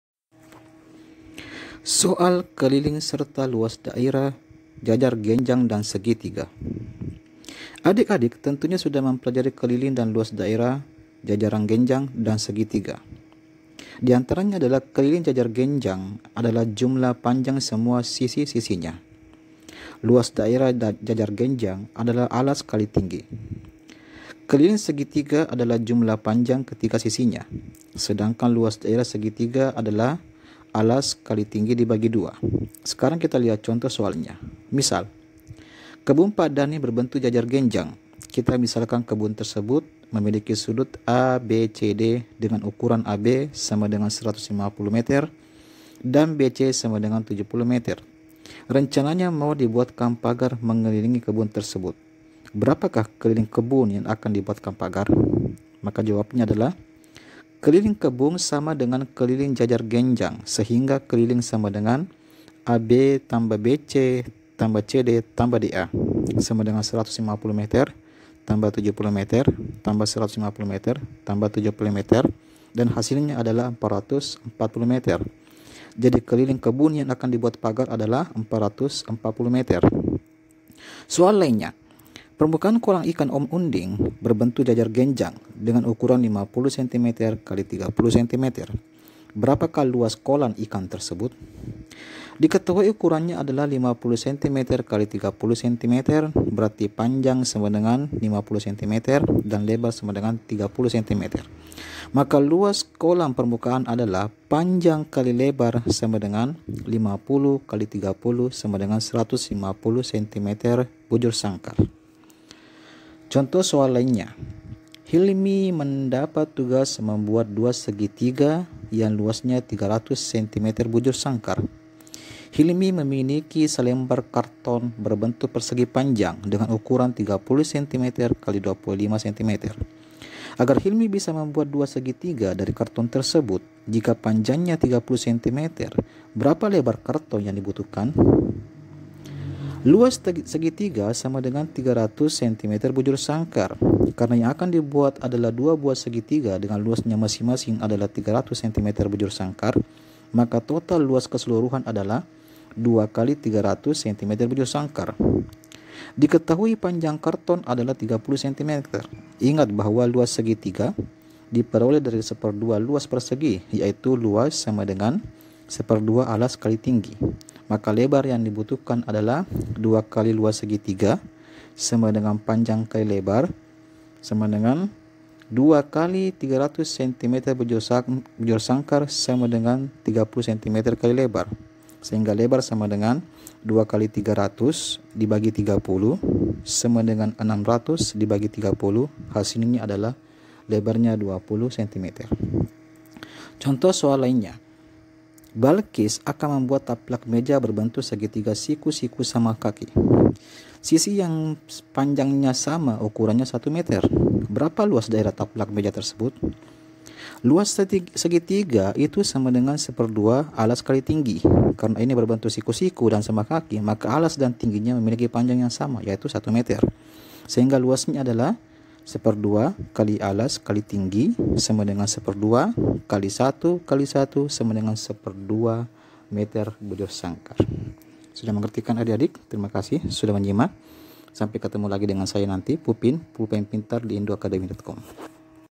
Matematika Kelas 4 SD - Bab 4 : Soal Keliling dan Luas Jajar Genjang dan Segitiga
MATERI PELAJARAN MATEMATIKA KELAS 4 SEMESTER 1 SD

BAB IV KELILING DAN LUAS DAERAH BANGUN DATAR SEDERHANA : Menyelesaikan soal keliling dan luas jajar genjang dan segitiga